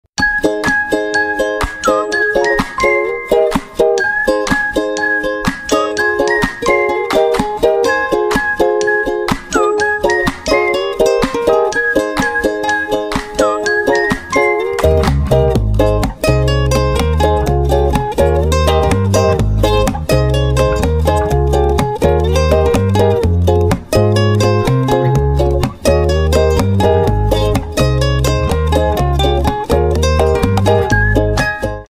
Reciprocating saws cut wood and metal
SHARP & EFFORTLESS CUTTING
 - Design of grinding teeth and dividing teeth
 - Conducive to improving cutting efficiency
 - Makes your pruning, wood cutting, and metal cutting smooth and effortless